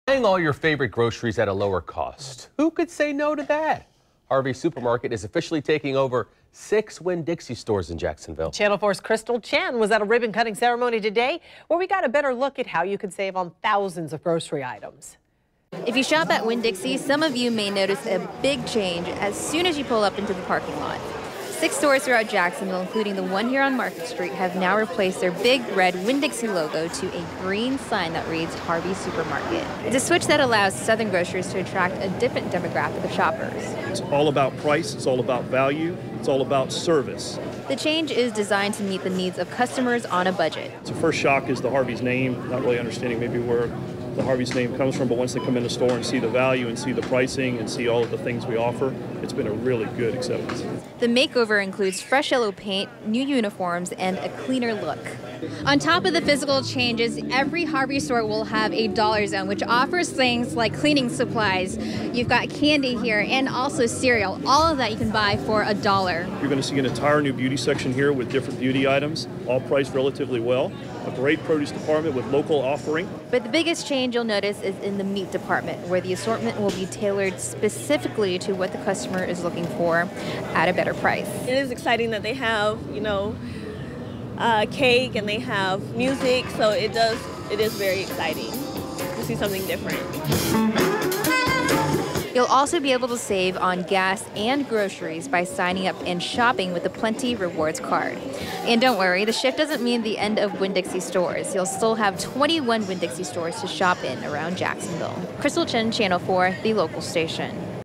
Harvey's Supermarket comes to Jacksonville
Harveys Supermarket is officially taking over 6 Winn Dixie stores in Jacksonville.